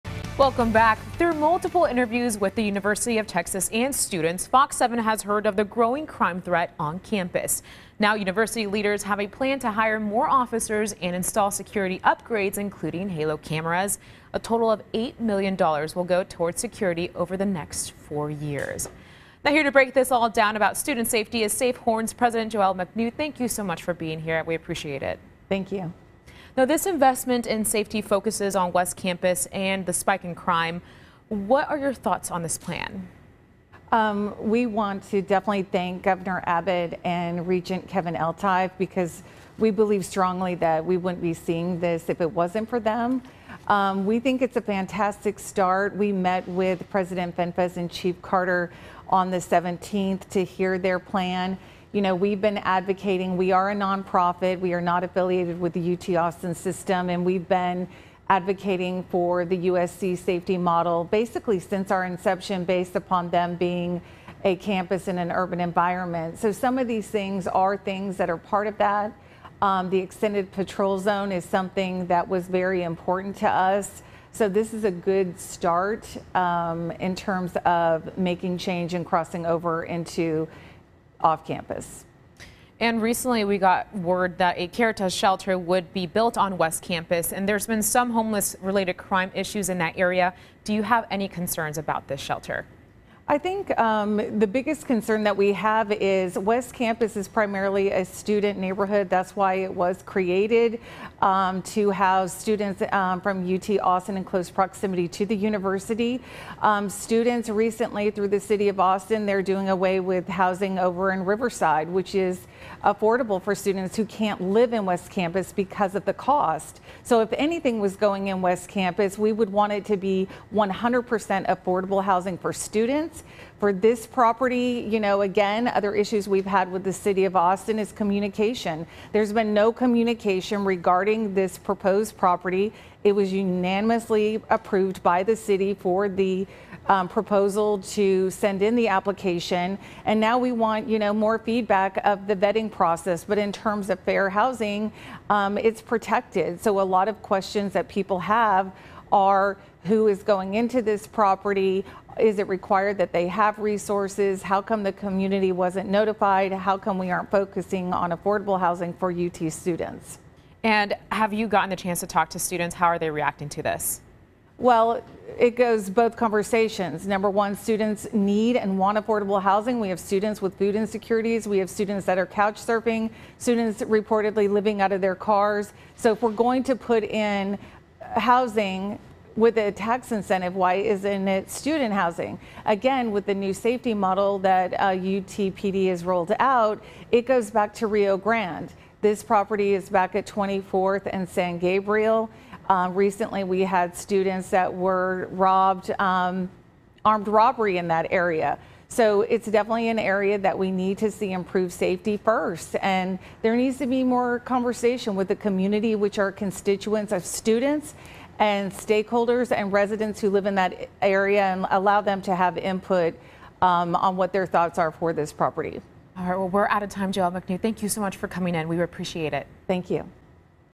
UT safety upgrades for West Campus
SafeHorns president Joell McNew joins Marcel Clarke to discuss UT Austin's plan to address campus safety.